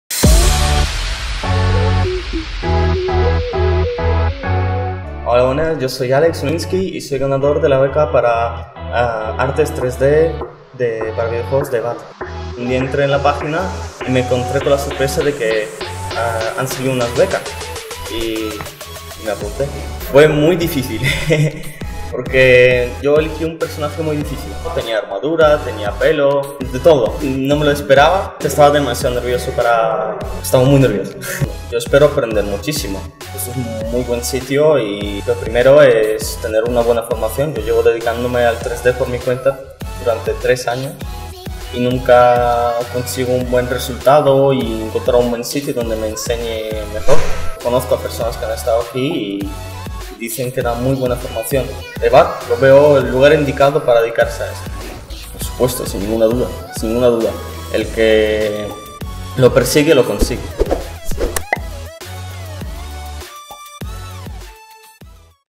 Entrevista a Alex Hulinskiy- Ganador de los Premios EVAD al Talento
Entrevistamos a Alex Hulinskiy, ganador de la I edición de los Premios EVAD al Talento en la rama de arte 3D.

Alex nos cuenta su experiencia en el concurso y cómo ve su futuro en EVAD  como profesional de la industria de los videojuegos, tras ganar la becade Premios EVAD al Talento para el Máster de Arte 3D y Animación para Videojuegos de EVAD.